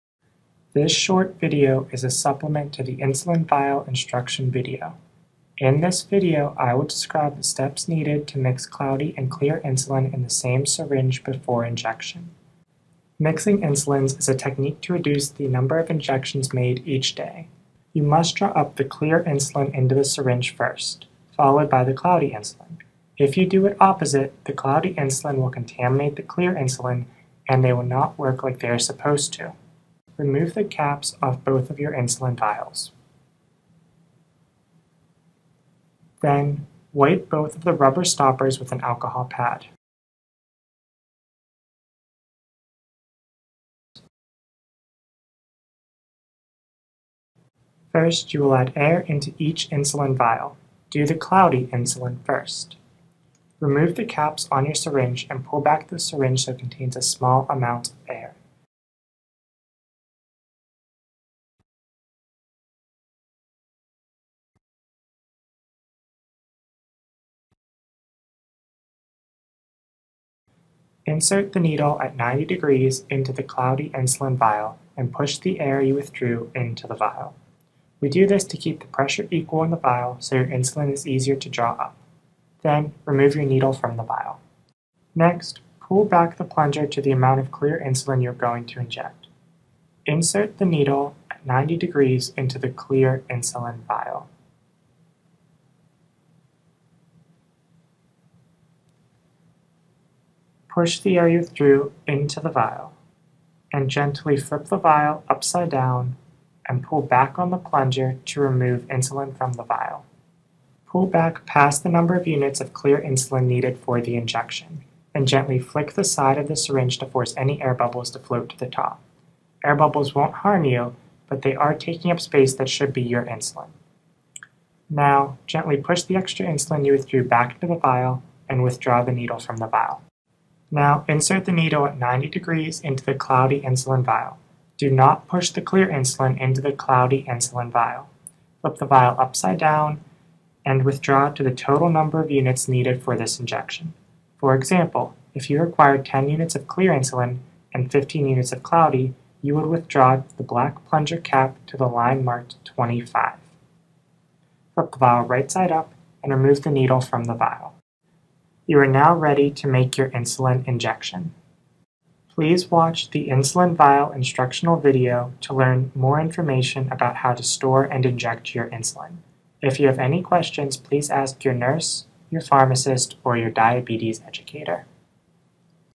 Mix Clear and Cloudy Insulin in SAME Syringe - UPDATED
This is a patient education video for patients who need to mix clear and cloudy insulin in the same syringe. It is a supplement to our "Insulin Vial Instruction" video.